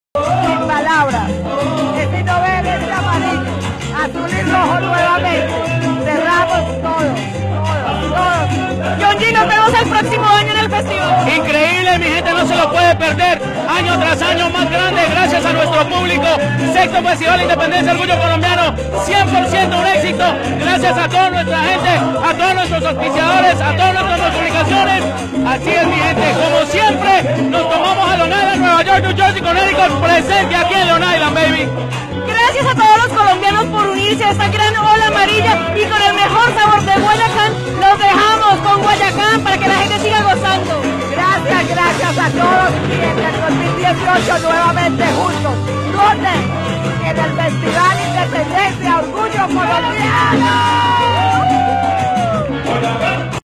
Colombianos se toman el Eisenhower Park en el Festival Orgullo Colombiano 2017
Una ola amarillo, azul y rojo se tomó el Eisenhower Park en el VI Festival Independencia